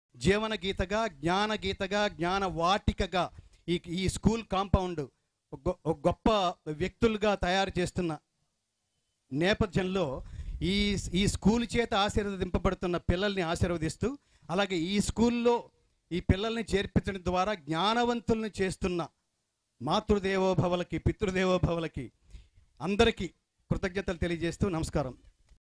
Gangadhara Sastry Praising Make My Baby Genius School
Gangadhara Sastry Speech at Make My Baby Genius School on the occasion of 300 students Reciting 100 Vemana Poems on 3rd,June 2018.